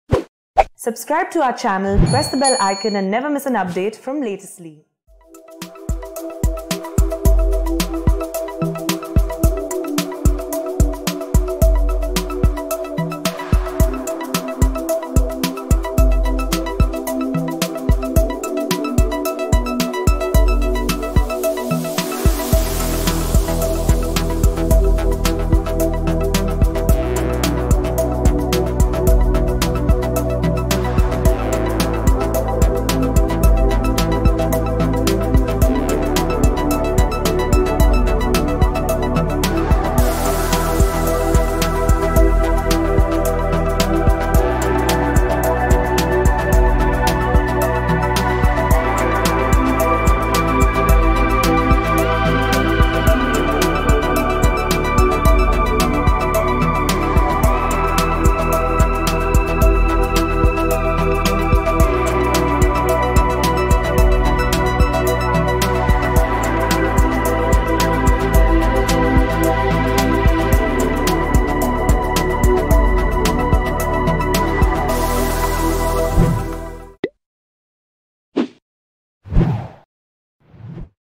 Lok Sabha Elections 2024: भाजपकडून 2024 च्या लोकसभा निवडणुकीची तयारी जोरात
देशात 2024 च्या लोकसभा निवडणुकीची तयारी जोरात सुरू आहे. लोकसभा निवडणुकीच्या पार्श्वभूमीवर भाजपने नव्या घोषवाक्याची घोषणा केली आहे, जाणून घ्या अधिक माहिती